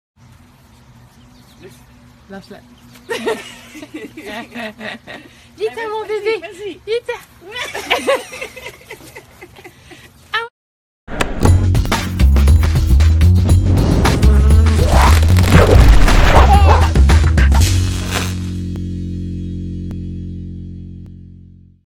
Aussie puppy refuses to walk in protective vest
Khira the Aussie puppy just flops over onto the ground after being fitted with a large and heavy protective vest. Too funny!